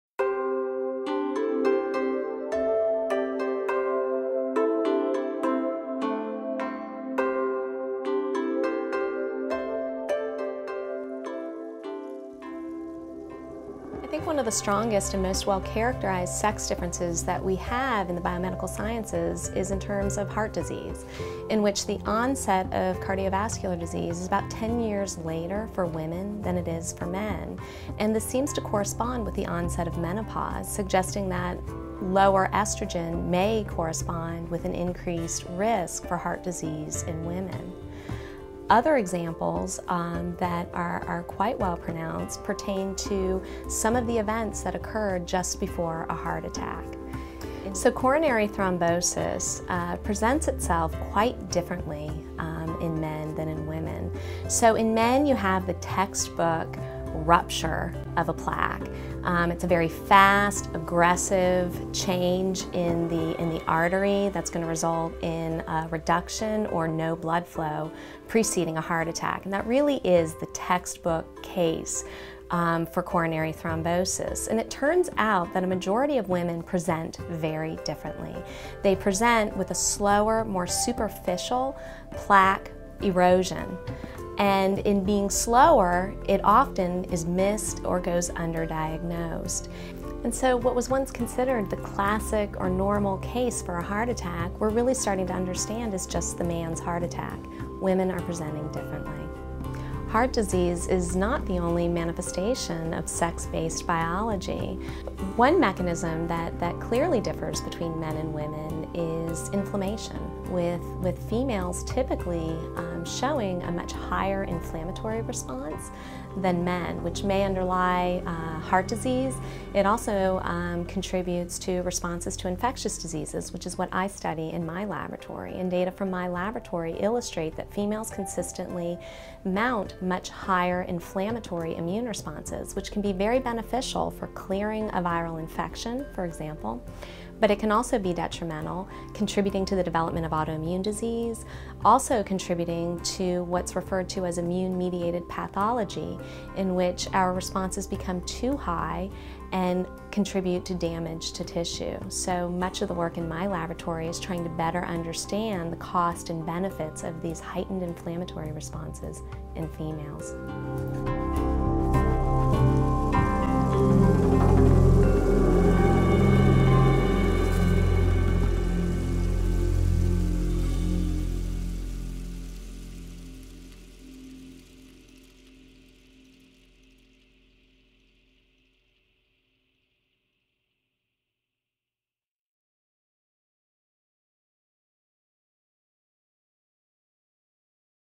Heart Disease Differences in Men and Women
Johns Hopkins Bloomberg School of Public Health researcher Sabra Klein explains how women's heart attacks differ from men's.